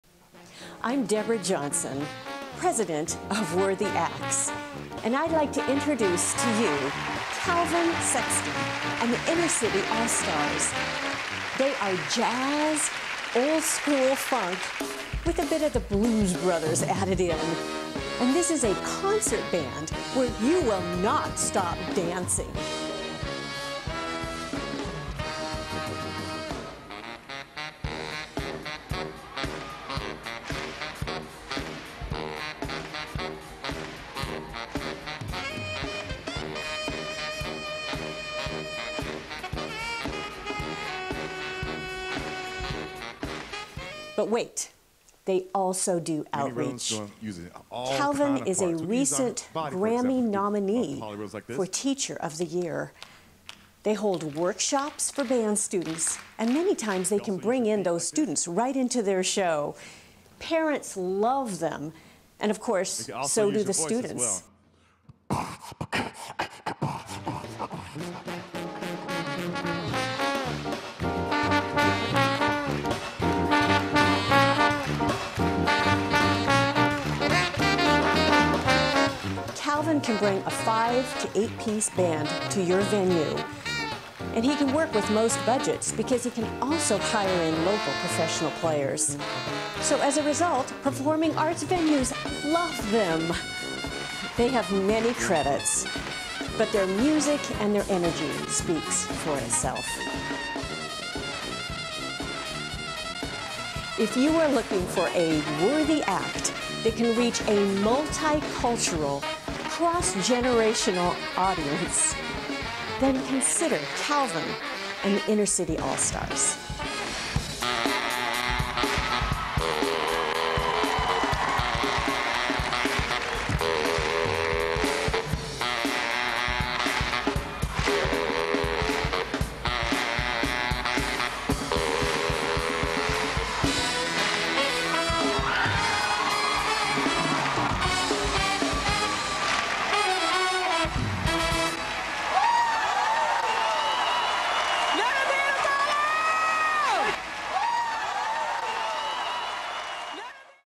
Calvin Sexton-Worthy Act
Calvin Sexton and the Inner City All-Stars is a mixture of jazz and old-school funk with a bit of the Blues Brothers! They are perfect for many Performing Arts venues as they not only provide a great concert where “You will not stop dancing” (Jonanna Widner, Dallas Observer) , but they provide great outreach for schools and communities! In fact, Calvin is a recent GRAMMY Nominee for Teacher of the Year!

Recent credits include: APCA “Best Band of the Year Winner,” National TV Performance for Showtime at the Apollo in NYC, Entertaining for the Armed Forces in Japan, Guam and Okinawa. They are also a Neighborhood Touring Program Roster Artist for the Dallas Office of Cultural Affairs and on the Texas Touring and Company roster.

Calvin can configure his touring band with everything from a 5-pc to an 8-pc band, even hiring in local professional players for Performing Arts venues, if needed.